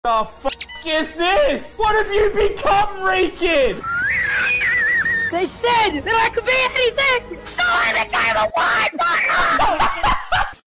so I became a wine bottle! |Gibson's first gift from Goldie|| Goldie's jacket
I don't know why I made this unit a whine bottle it was supposed to be Gibson and Goldie's jacket XD